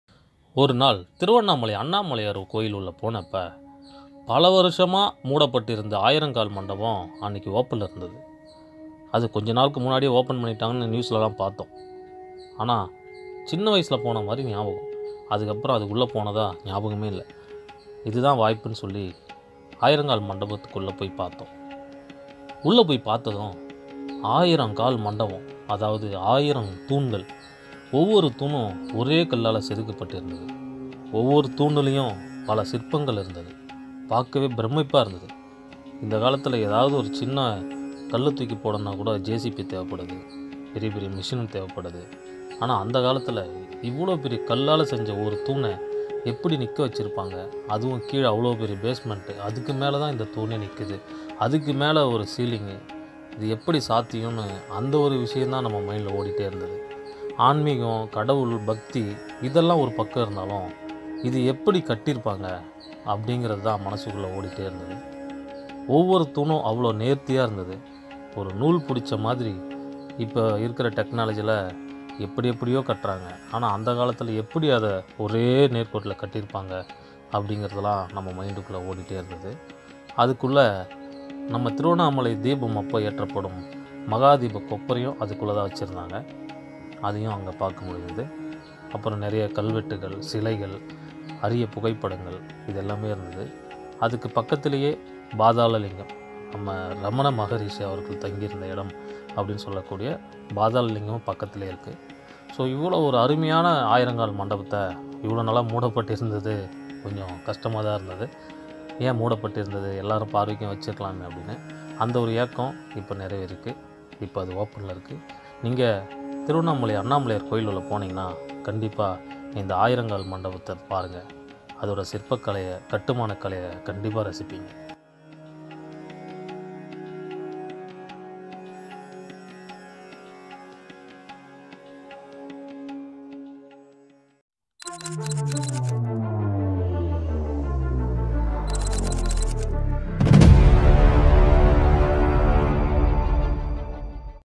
திருவண்ணாமலை பெரிய கோயில் போனா இதை மிஸ் பண்ணாதீங்க | ஆயிரம் கால் மண்டபத்தின் அற்புதங்கள் | TT
more videos about TIRUVANNAMALAI...